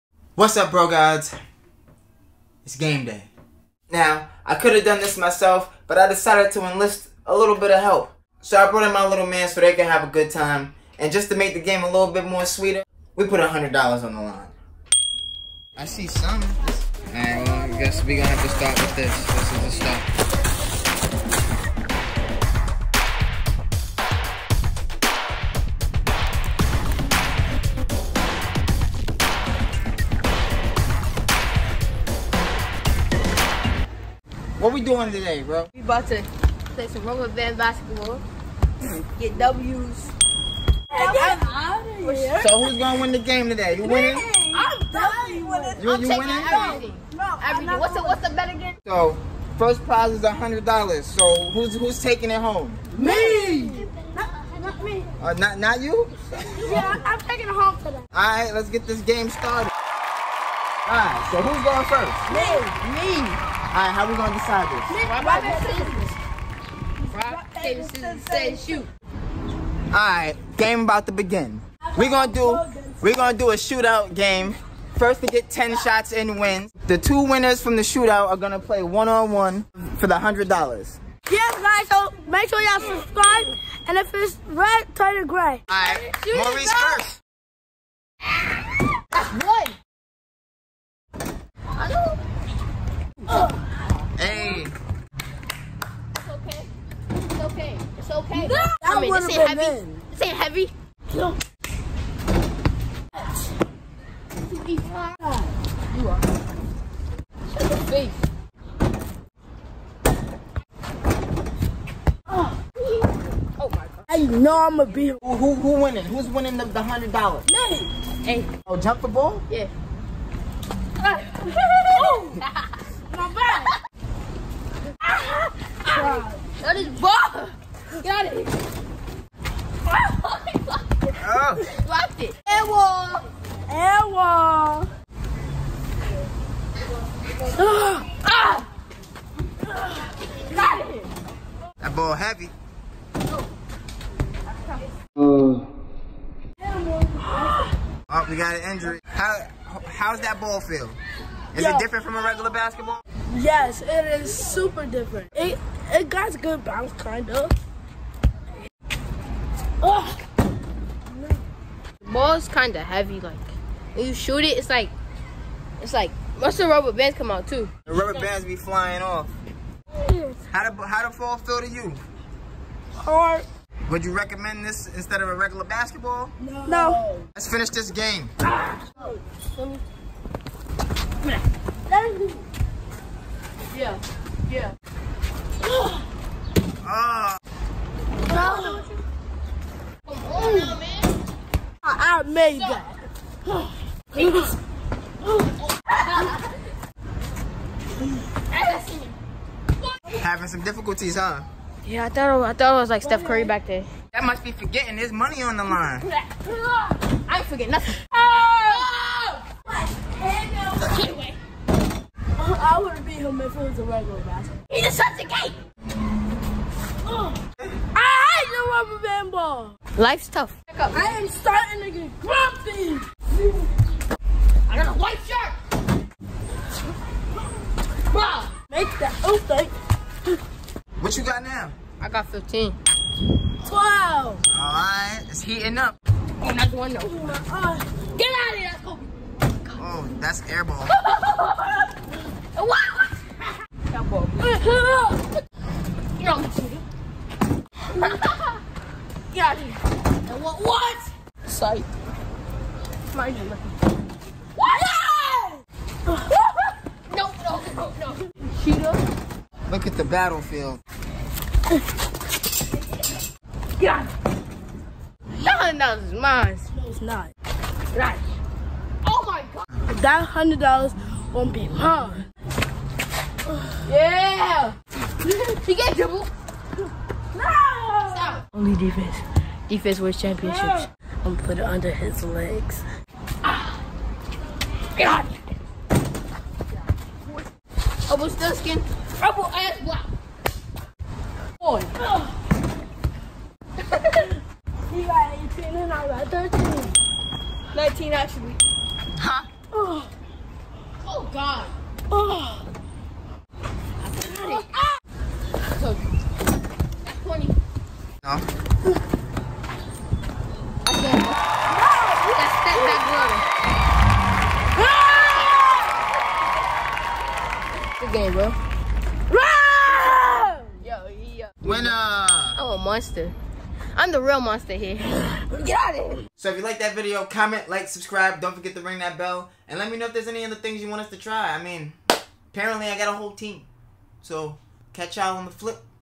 Rubber band Basketball ($100 Game )
My little mans play a game of 21 with a Rubber band basketball for $100